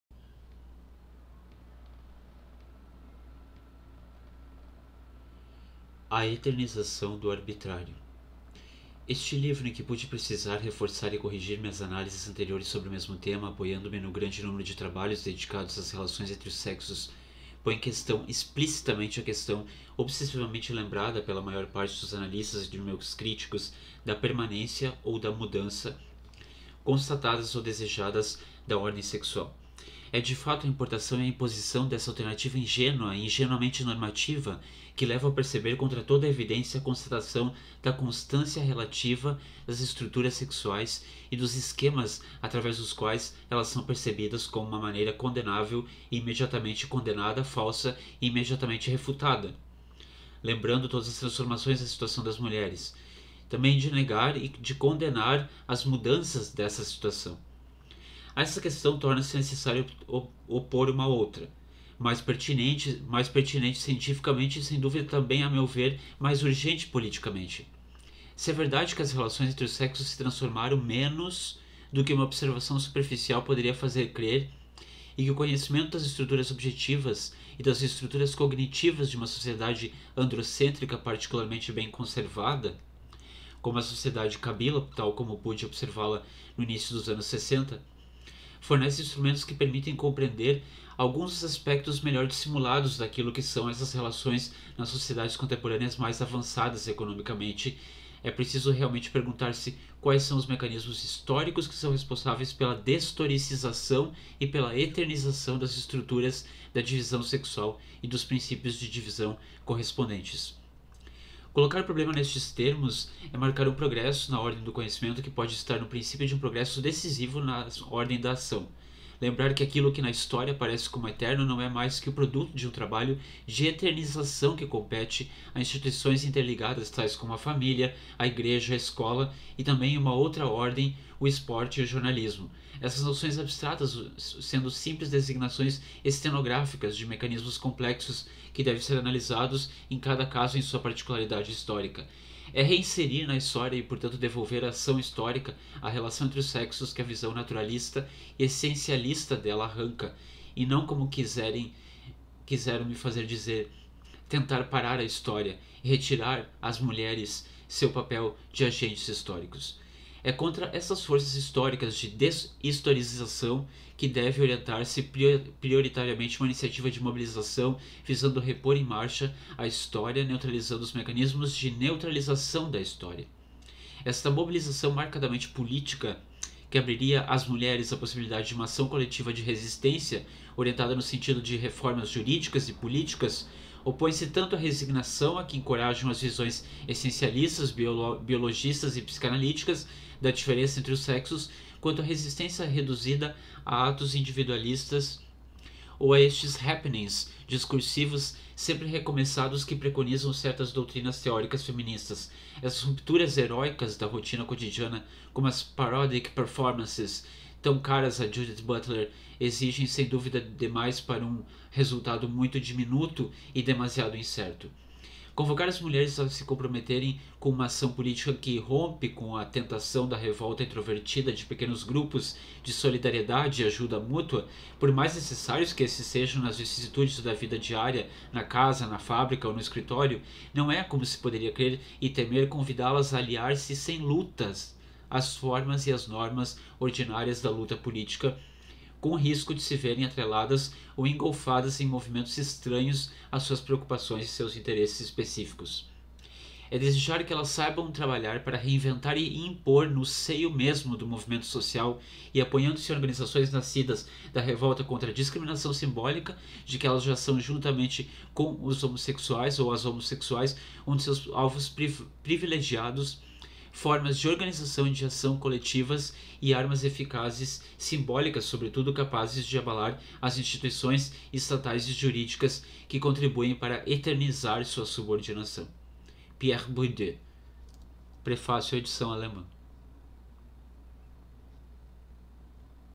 A dominação masculina - Pierre Bourdieu [Prefácio à edição alemã]
Prefácio à edição alemã de A dominação masculina de Pierre Bourdieu lido por G R TISSOT
Tradução de Maria Helena Kühner